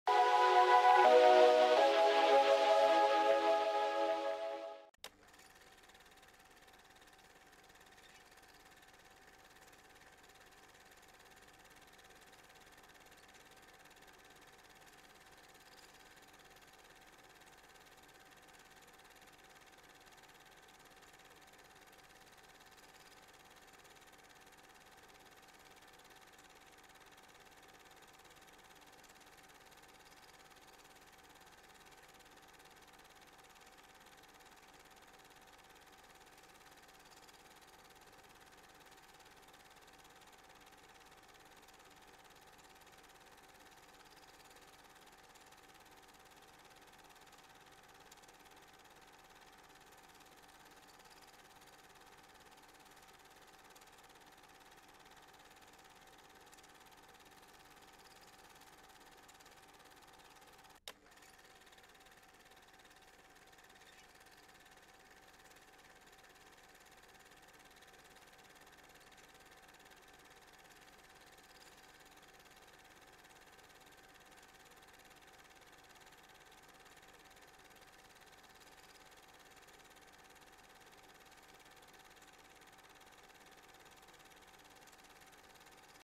Thủ tướng CHXHCN Tiệp Khắc Jozef Lenárt thăm miền Bắc Việt Nam năm 1966
Jozef Lenárt (3 tháng 4 năm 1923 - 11 tháng 2 năm 2004) là một chính trị gia người Slovakia, từng là Thủ tướng của Tiệp Khắc từ năm 1963 đến năm 1968.